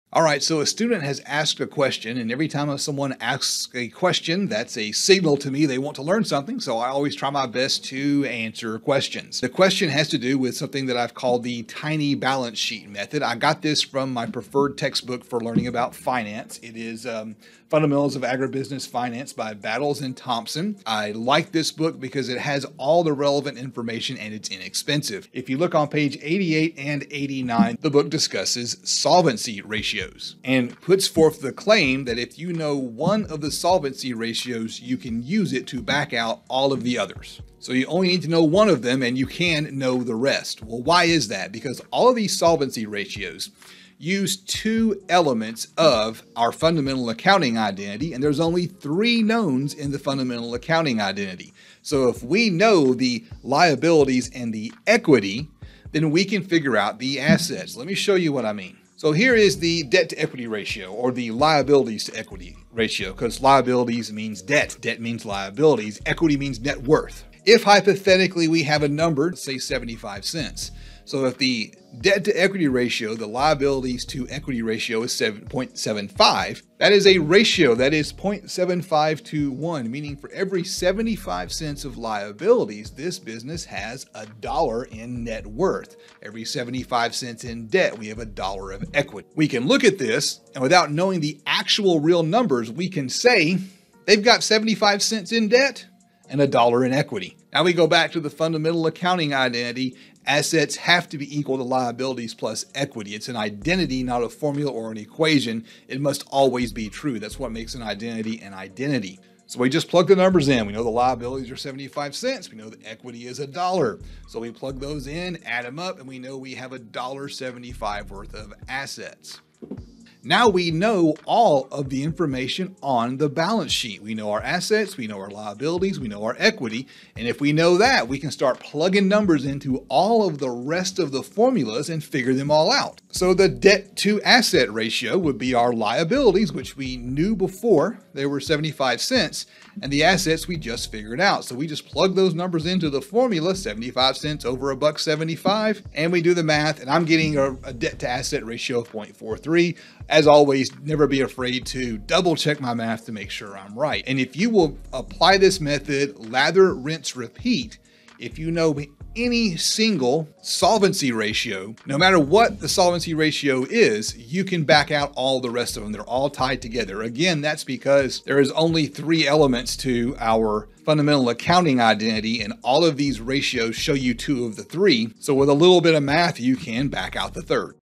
How to calculate Solvency Ratios!
In this video we learn that if you have one of the three solvency ratios you have enough information to know the other two.

Photo credit:  Cow by Harry Wubbles